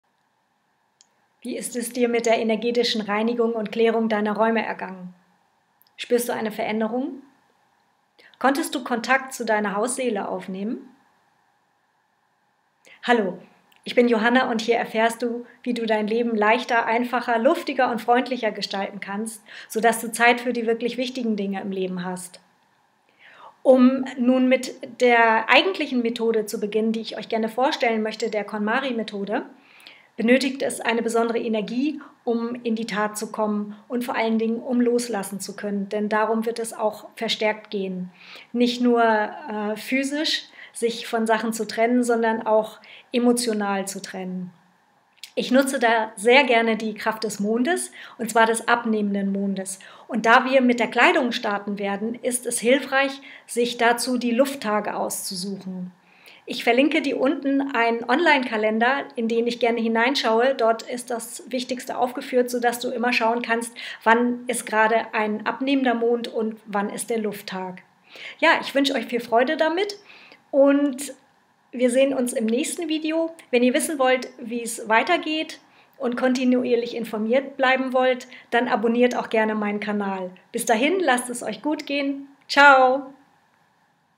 Wohnung Räume aufräumen ordnen - unterstützende Mond Energie
Nutze die positive Energei des abnehmenden Mndes, um leicht mit dem Aufräumen und Ausmisten zu beginnen. So bringst du Ordnung in dein Leben, um wieder mehr Zeit für's Wesentliche zu haben.